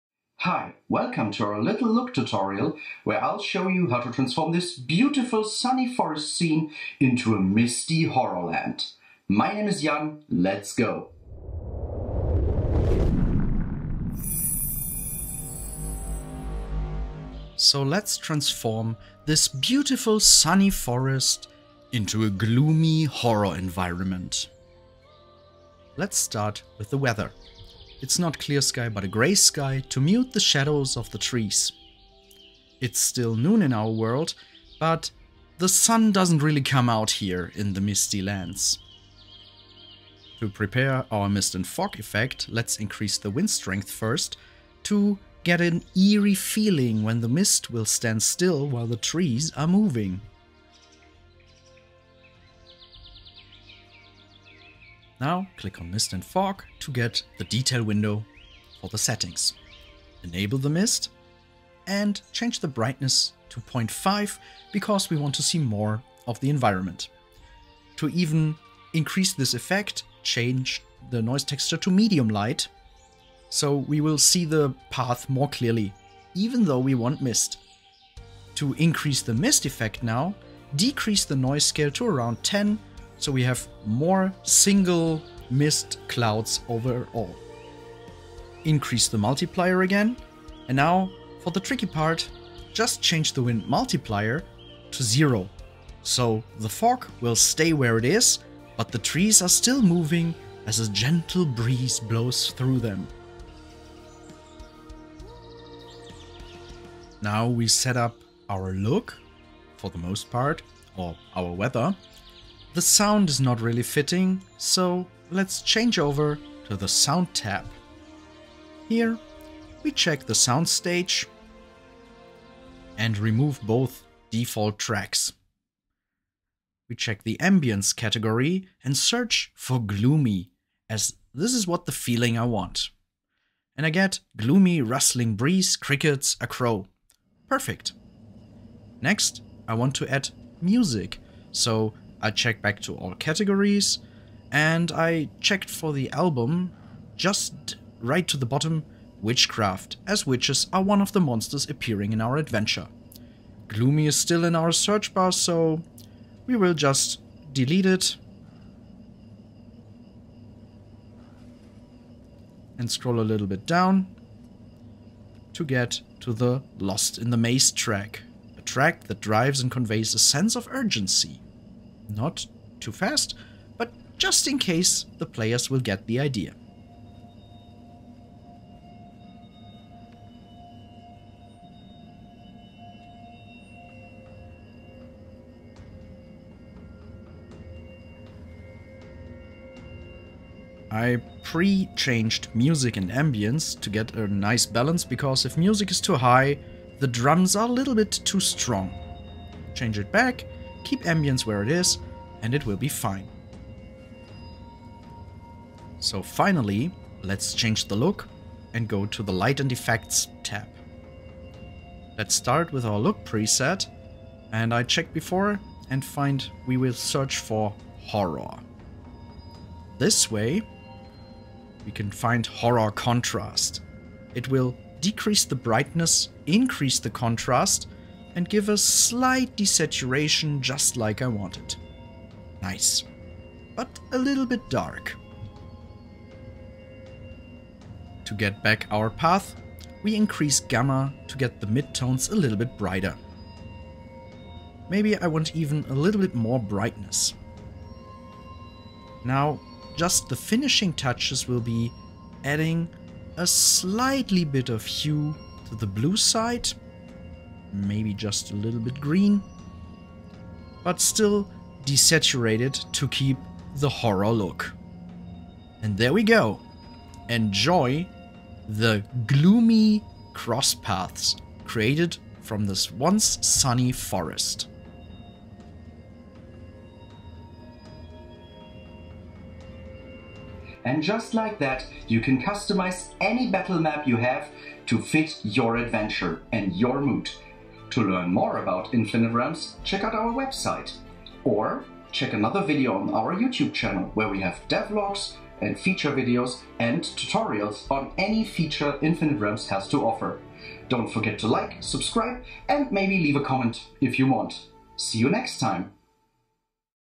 From Sunny to Sinister with Quick & Easy Battlemap Customization!
You found just the right place for your next encounter, but the players took until evening to reach this place and now the map doesn't fit right? Or the ruins are exactly what you were looking for, but a bit more mist and lightning would make it perfect? In this little look tutorial we show you, how you can customize any map to your hearts desire.

We start with one of our 3D maps. "Crossing Paths" was designed to be just that: a lot of paths through a forest, from north to south, east to west, crossing here and there, to have just the right place you are looking for. By default the sun is shining and we have a clear sky, so we see the shadows of the trees. As we want to create a misty way through a cursed forest, "Grey Sky" is our weather preset to go. The next thing we change, is the wind, to move the trees a bit more than they are by default. Then the mist is added and tweaked, so we can still see the path and the mist doesn't move with the wind, because it should be a cursed horror forest.

Another important feature, at least for us, is the sound. A good ambiance track and fitting music can enhance the immersion even more. So we remove the default sounds and music, as they are way too happy. A great thing in Infinite Realms are the filters and search bar. We added so many sounds and music tracks, that finding the right one would be difficult without it. Once the tracks are added we finish this off with the right volume and call it done.

Last but not least we switch over to the Lights & Effects tab. This is where we put the finishing touches on our desired look. The Look Presets also have filters and a search bar, just like the sounds, so you can easily find the right look for your mood. Here we pick the "Horror Contrast" to make everything darker and a bit more pale. With the brightness and gamma slider we fine tune the image, so it doesn't get too dark. A slight shift to the hue and a bit less saturation is all we need to finish our desired look.

And just like that, we transformed a once sunny forest into a spooky, misty, horror land. It just took five minutes, so even if your players are much too late or change directions, you can make these changes on the fly.

To learn more about Infinite Realms and the features it offers, check our